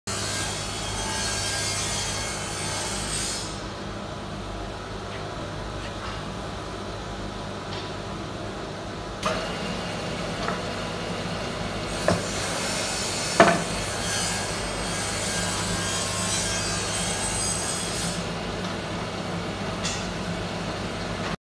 Making traditional yosegi zaiku woodworrk veneer by super surfacer machine
I had this old machine more than 4 yrs. recently had a chance replaced the conveyor belt. It looks working perfect!